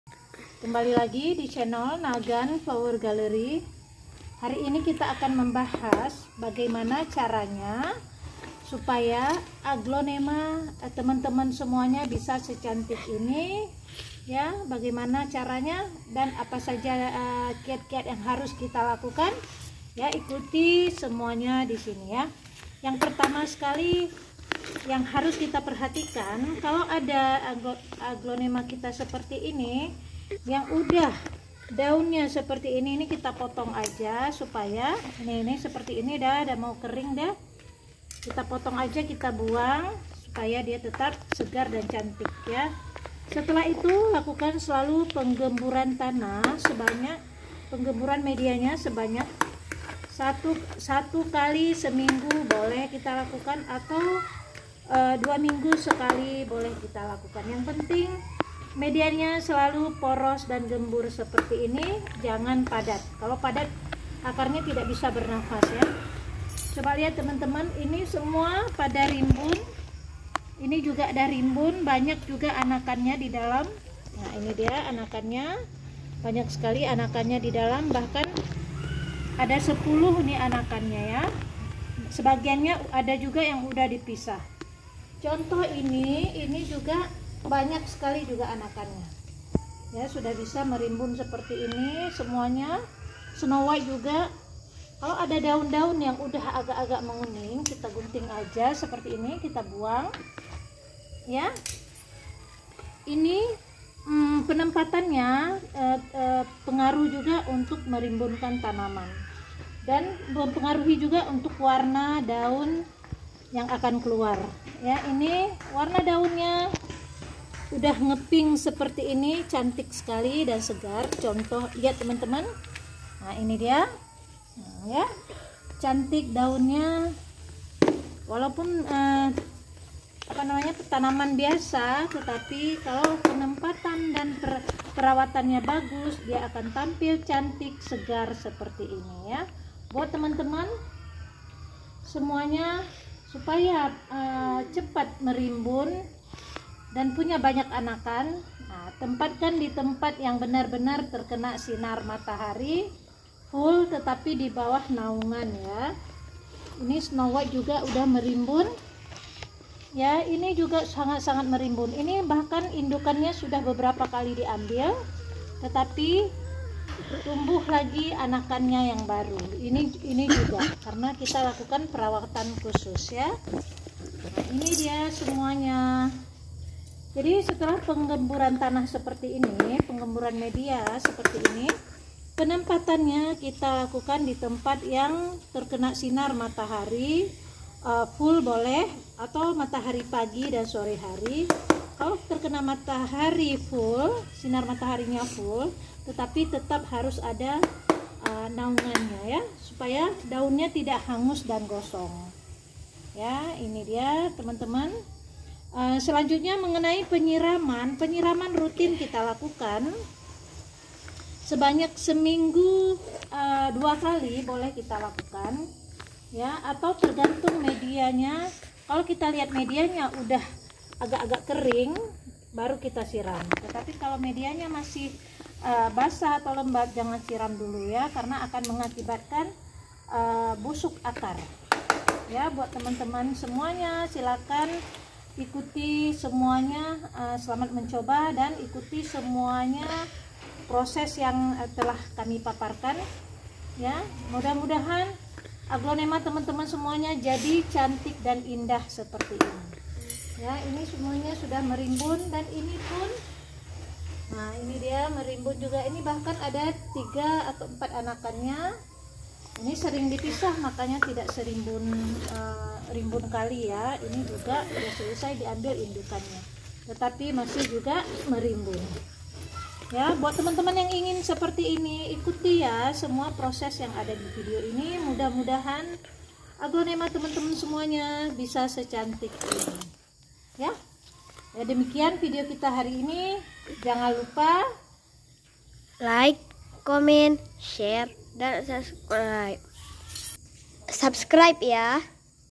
BAGAIMANA SIH CARA MEMBUAT AGLONEMA AGAR SEHAT DAN MERIMBUN?
hai semuanya...kali ini kami akan memberitahukan kainnya. supaya aglonema kalian bisa seperti saya. dadahhhh...